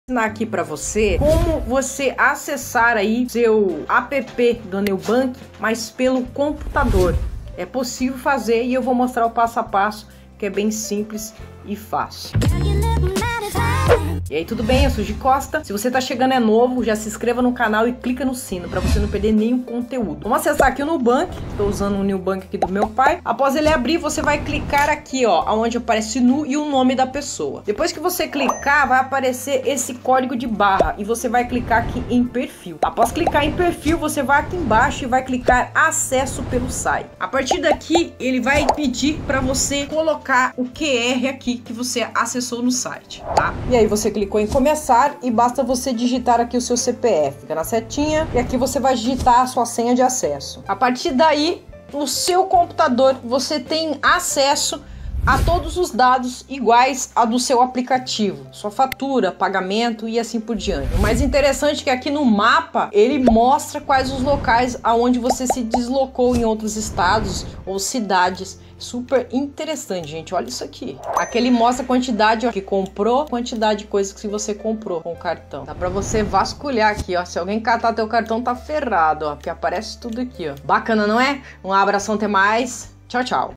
DESCUBRA ONDE COMPRARAM COM SEU CARTÃO NUBANK
Hoje quero ensinar a usar o app do Nubank tanto pelo app quanto pelo pc, e o melhor de tudo é saber onde seu cartão foi usado. No site do nubank aparece um mapa onde mostra cidades onde você comprou com o seu cartão nubank e o nome da loja e endereço, é muito interessante. Caso alguém pegue seu cartão você poderá saber onde e qual cidade a pessoa comprou com seu cartão nubank.
Acompanhe o passo a passo em nosso vídeo!